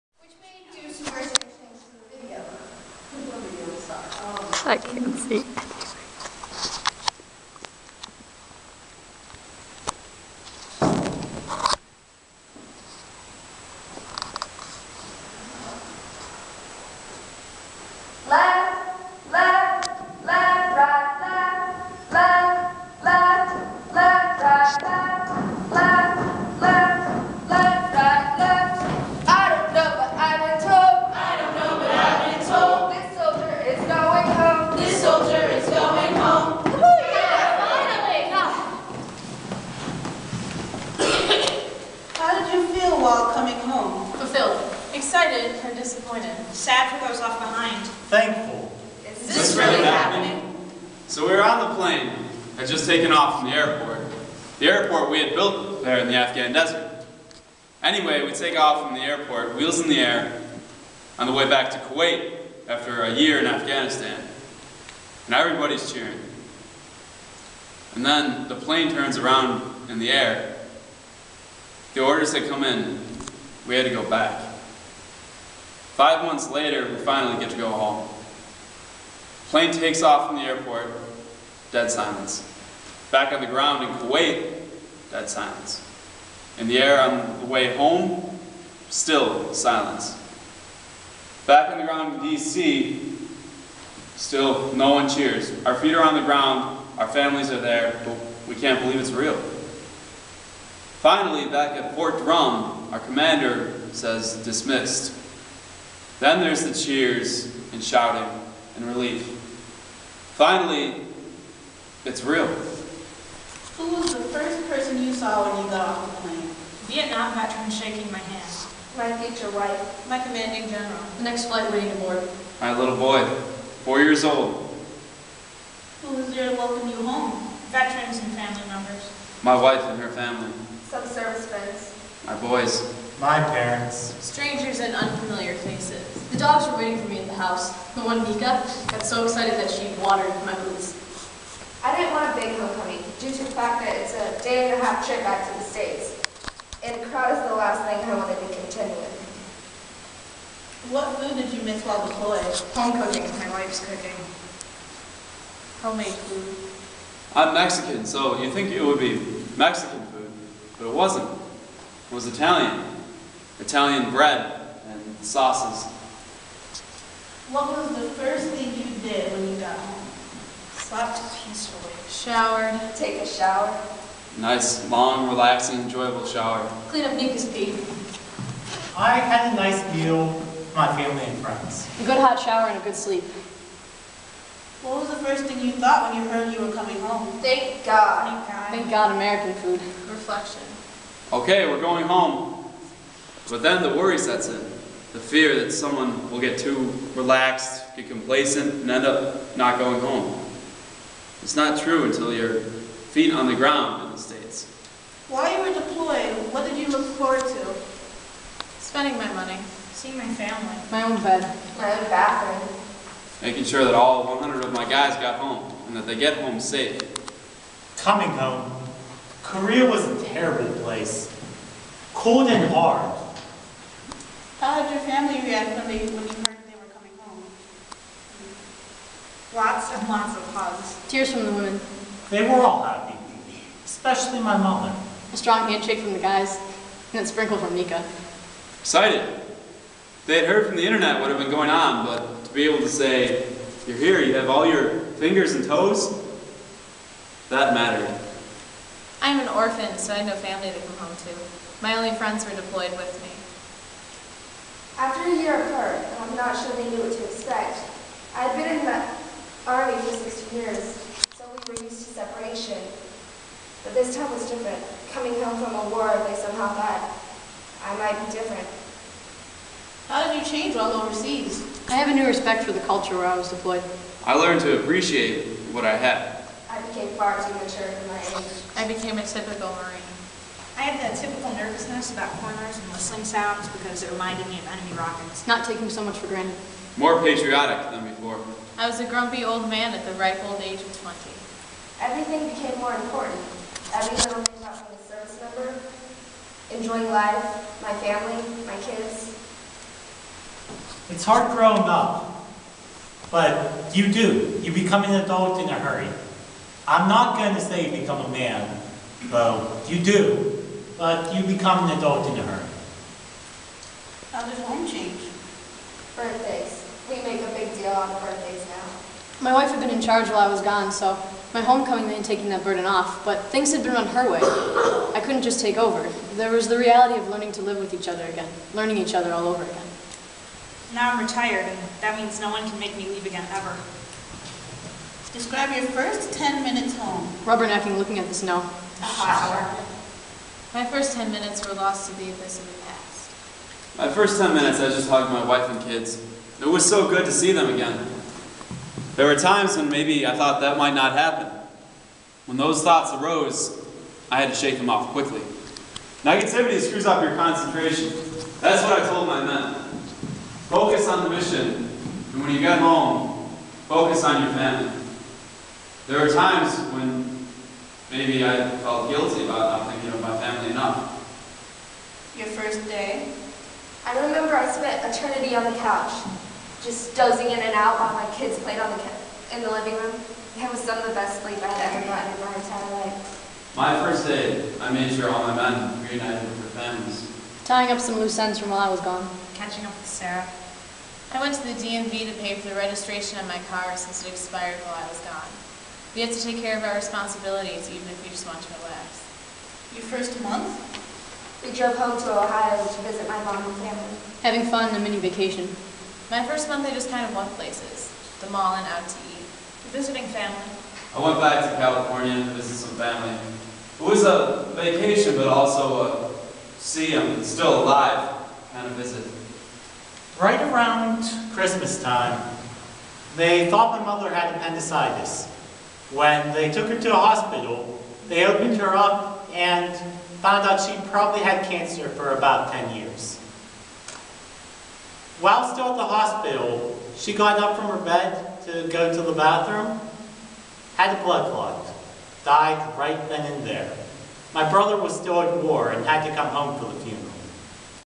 SBU Theater - Coming Home (Part 1 of 2)
Members of St. Bonaventure University's Theater Department wrote and performed this piece, entitled "Coming Home" for the Kennedy Center American College Theater Festival Region 2 for the Devised Theater exhibition. The prompt was "Welcome Home." The production debuted at the festival in January 2012, where it was well received. This taping is from an open dress rehearsal a few days before the festival at St. Bonaventure University. Directed by Professor Rebecca Misenheimer, the cast includes: Mary Best, Mike Dlugosz, Will Foust, Becky Hahn, Tori Lanzillo, Makeda Loney, Rebecca Misenheimer, Brooke Perkins and Emily West.